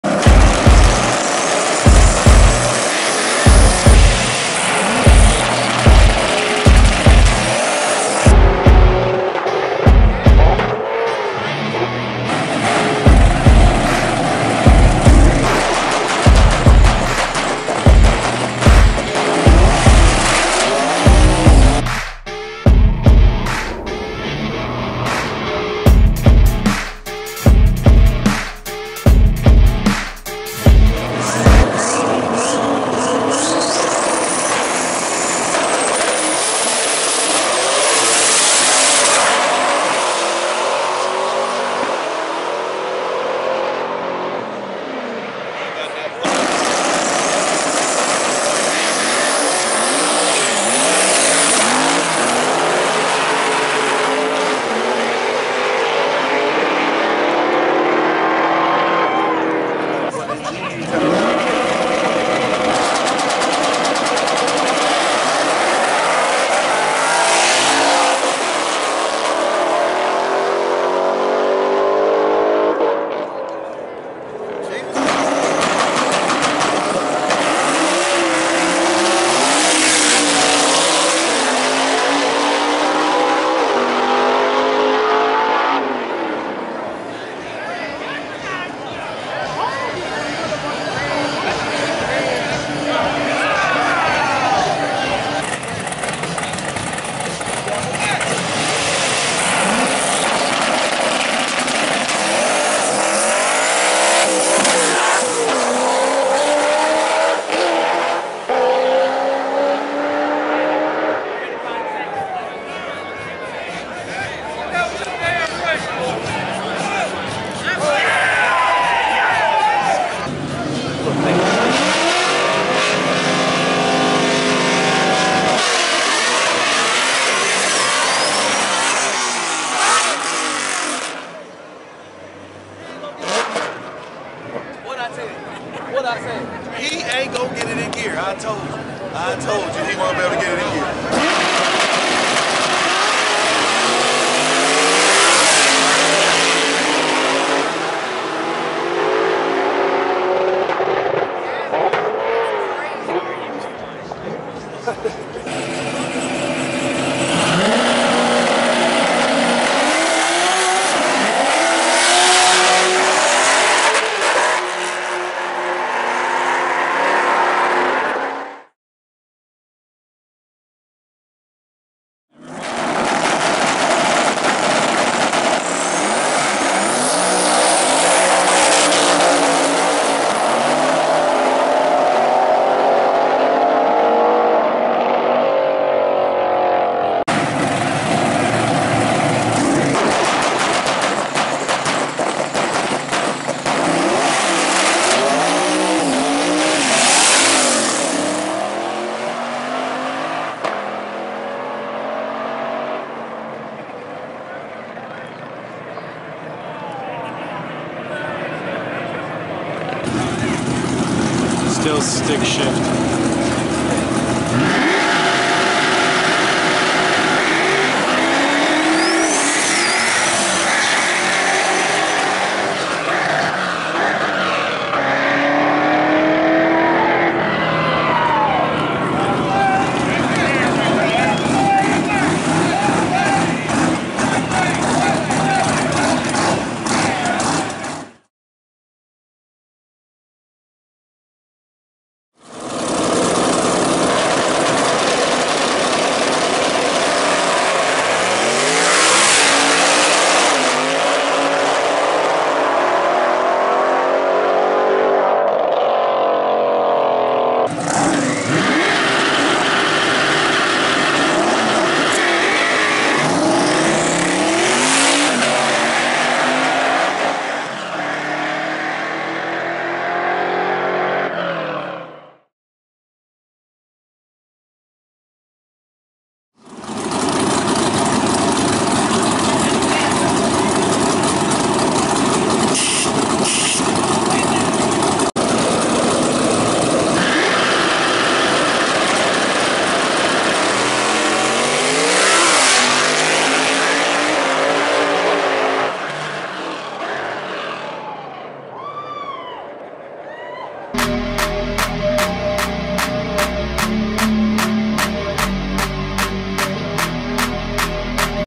Dig or Die: Stick Shift class
THIS VIDEO STICK SHIFT TAKE CONTROL AND A LOT OF THE RACES DID NOT GO ACCORDING TO PLAN!

WITH THE DRIVERS MISSING GEARS, SPINNING ON THE LINE, NITROUS BACK FIRES, AND JUST BEING SLOW, A LOT OF DRIVER LOST BEFORE THEY EVEN BEGAN!

BUT YOU KNOW WHAT?!?! I HAVE TO GIVE IT TO THEM BECAUSE GOING OUT THERE BANGING GEAR IN FRONT OF A CROWD AND TRYING TO NOT LOOK LIKE AN IDIOT IS NOT AN EASY TASK LET ALONE GO AGAINST SOMEONE IN THE OTHER LANE!

I REALLY HOPE TO SEE THIS CLASS GROW AND WE SEE SOME BADASS STICK SHIFT CARS IN THE FUTURE BECAUSE THESE DRIVERS ARE IN A LEAGUE OF THEIR OWN!